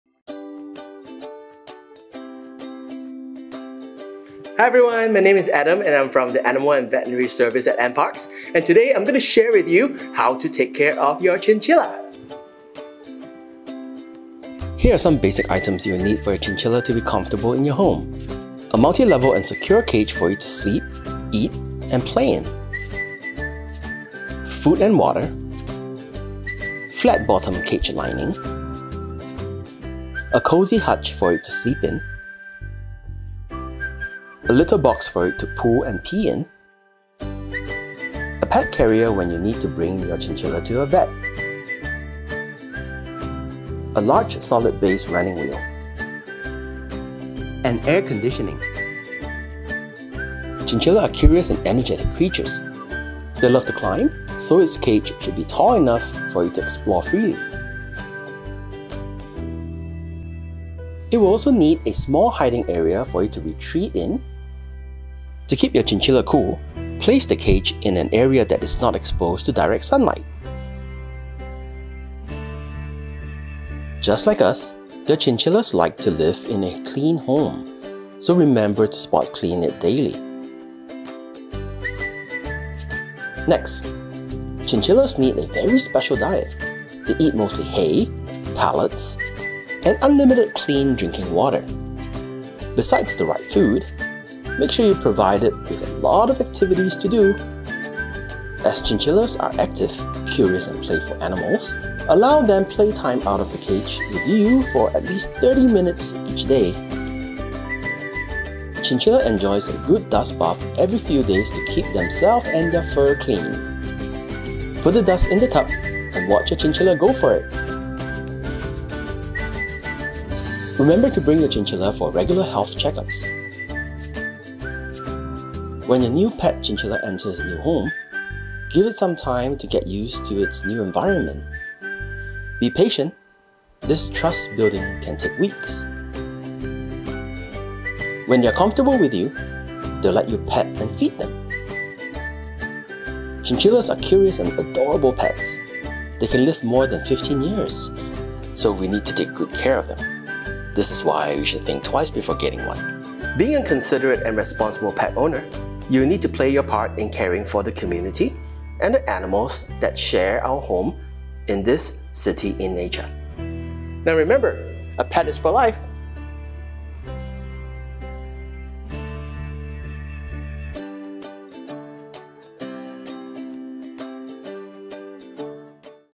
Basic Pet Care: Chinchilla
Chinchillas are adorable but they require constant care, commitment and resources. Learn how you can provide a comfortable and caring home for your pet chinchilla in this video!

Follow NParks and AnimalBuzzSG on social media!